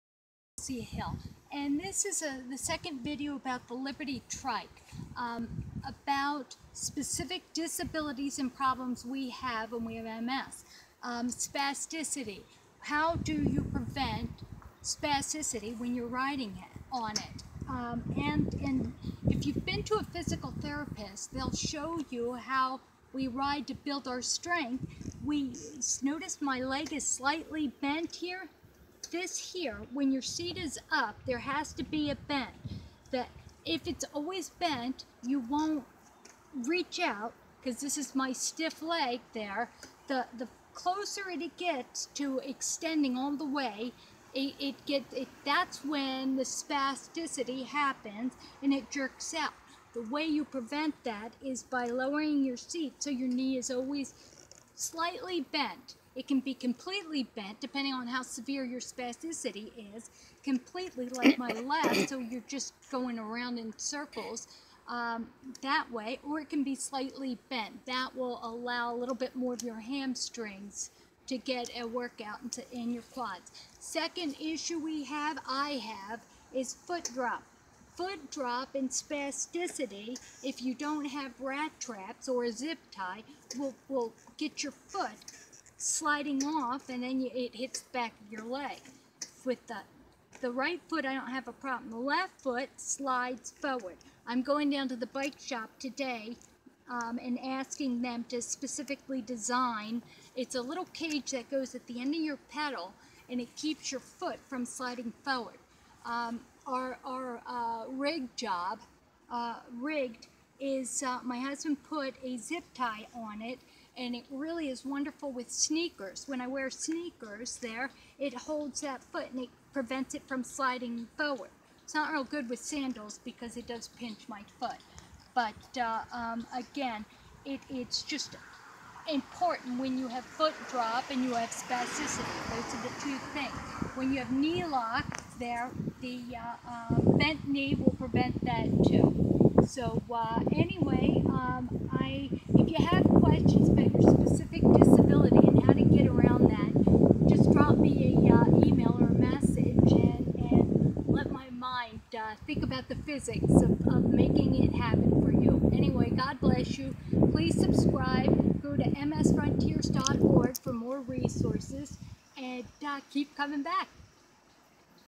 Adapting to MS limitations on your trike
Spasticity, knee lock, and foot drop are all elevated with simple adjustments.